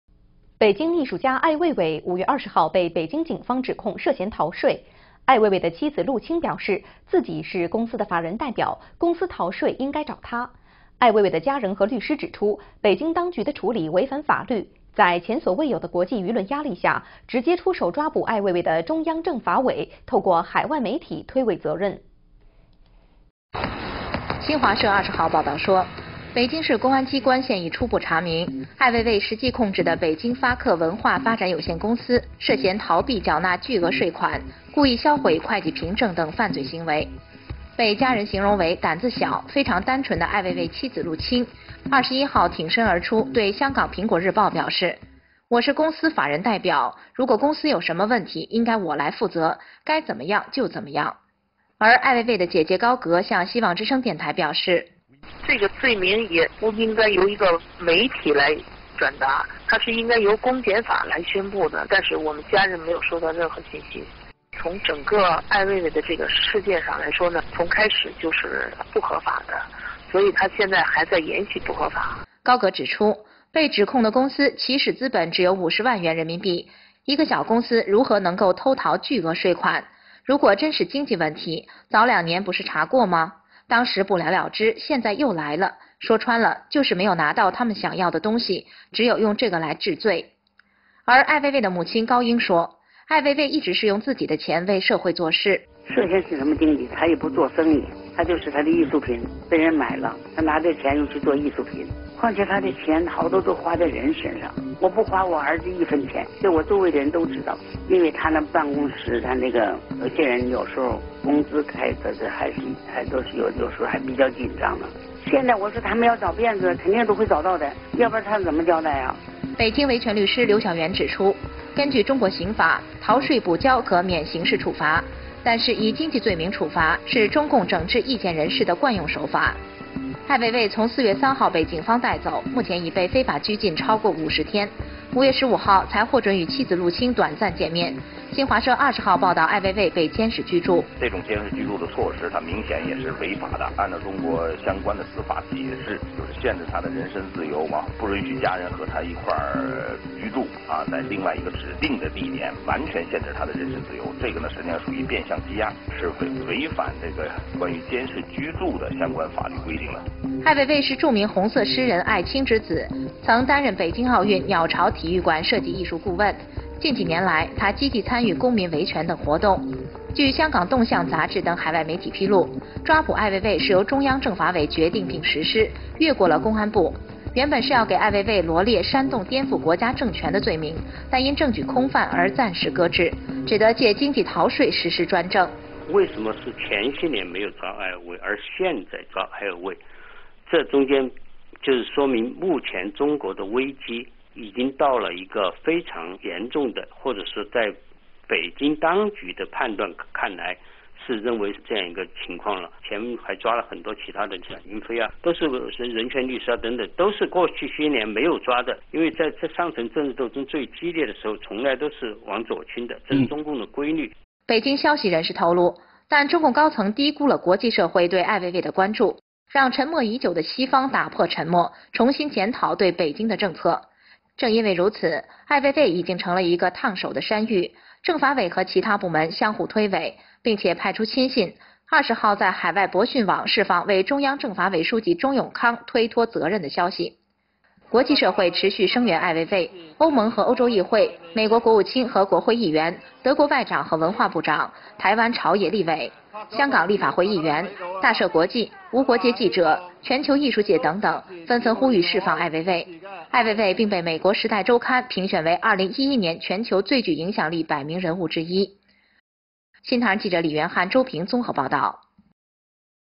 Ai Weiwei's Wife Refutes Allegations
Beijing artist Ai Weiwei was accused of tax evasion
on May 20. Ai's wife, Lu Qing said:
"I am the legal representative of the company
and should be responsible." Ai's family and lawyer
pointed out that Beijing authorities breached laws.
The Central Political Committee is receiving
an unprecedented international pressure.

Chinese Communist Party (CCP) mouthpiece,
Xinhua News Agency reported that Beijing
public security authorities said Ai's Beijing Cultural
Development Co., Ltd. allegedly
"avoided paying huge taxes
and intentionally destroyed accounting vouchers."

Described by the family as "timid, very simple,"
Lu Qing, told Apple Daily on May 21:
If the company has any questions to answer,
I should be responsible."

Ai's sister Gao Ge told Sound of Hope Radio:
"The charges should not be conveyed by media
but the public security. Our family did not receive
any information. Ai's arrest was illegal
from the beginning, and it is still illegal now."

Gao Ge said Ai's company's start-up capital
was only RMB500,000, how could it evade
"huge taxes"? If this is only an economic issue,
why has it not been checked two years ago?
CCP did not get what it wanted,
and now only claims such illegalities as punishment.

Ai's mother Gao Ying, said Ai used his own money
to do things for the community.

Gao Ying: "He did not get into economy at all
and does not do business. His artwork was bought,
and he used the money to do more artworks.
His money was spent on others, I did not get any
from him. People around me know his office
could not pay others well. If they want to find flaws,
they will, or else how could they report upwards?"

Liu Xiaoyuan, Beijing human rights lawyer
pointed out that under the Chinese criminal law,
tax evasion could be exempt from punishment.
Economic penalty is one of CCP' tricks on dissidents.

Ai was taken away on April 3,
and is illegally detained for over 50 days now.
On May 15, Ai was allowed to meet with his wife,
Lu Qing. Xinhua News reported on May 20
that Ai was under residential surveillance.

Mo Shaoping, Beijing human rights lawyer, said:
"Residential surveillance is clearly against the law.
Limiting his personal freedom, not allowing family
to live with him and restricting his personal freedom
somewhere else, are all various methods
of detention. It violates laws."

Ai is the son of famous poet Ai Qing, and is a former
art consultant of Beijing Olympics' "Bird's Nest"
stadium design. In recent years, he was actively
involved in civil rights and other such activities.

According to HK Trend magazine, the arrest of Ai
was decided and implemented by Central Political
and Law Commission. They originally intended to
charge Ai with "inciting subversion of state power",
but it was shelved due to lack of evidence.
Eventually they charged Ai with a tax evasion.

Chen Kuide, China Initiative Executive Chairman
in Princeton said: "Why did they not arrest Ai
a few years ago, but only arrest him now?
It is because the current crisis in China
is very serious to Beijing authorities.
Others like Ran Yunfei, and human rights lawyers
arrested now, were not getting arrested before.
When political struggle gets furious,
it's usually left-leaning, to the CCP."

According to Beijing sources, CCP underestimated
the international society's focus on Ai,
which broke the long silence of the western world
and made them review Beijing's policies again.
Now Ai has become a hot potato,
members of the Politics and Law Committee
and other departments are buck-passing.
On May 20, articles on Boxun Web were shirking
responsibilities for Zhou Yongkang, secretary
of the Central Political and Law Commission.

The international society continued to support Ai,
EU and European Parliament,
U.S. Secretary of State and congressman,
German Foreign Minister and Minister of Culture,
and many other organizations
have called for the release of Ai Weiwei.
Ai is in the "Top 100 Most Influential People for 2011."

NTD reporters Li Yuanhan and Zhou Ping